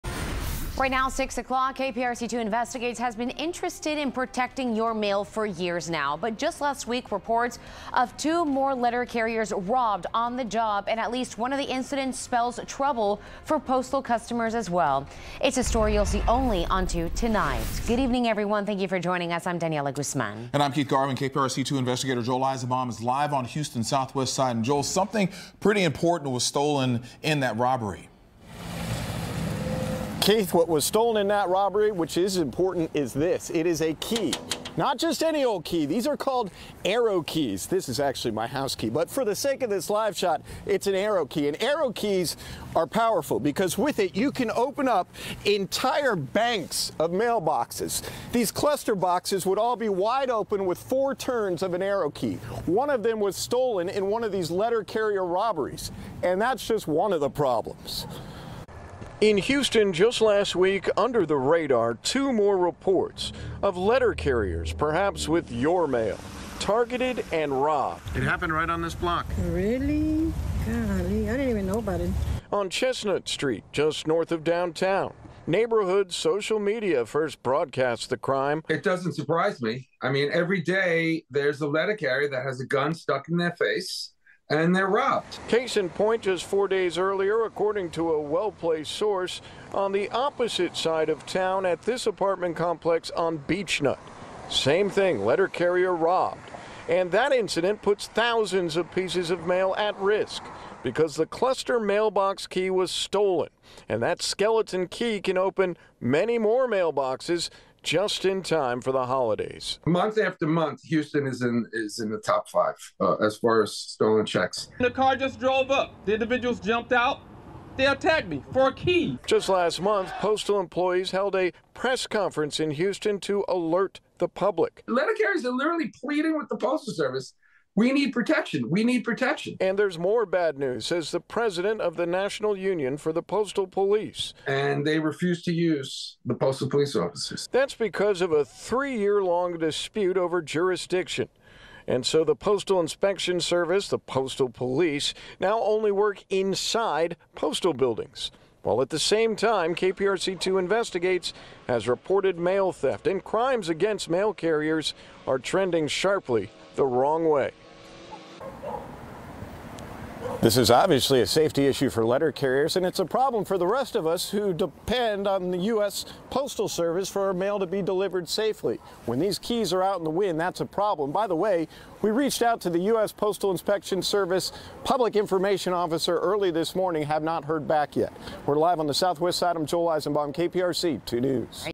Houston USPS letter carriers targeted in robberies
Last week, letter carriers on the job in Houston were the victims of armed robberies, according to reports from multiple sources.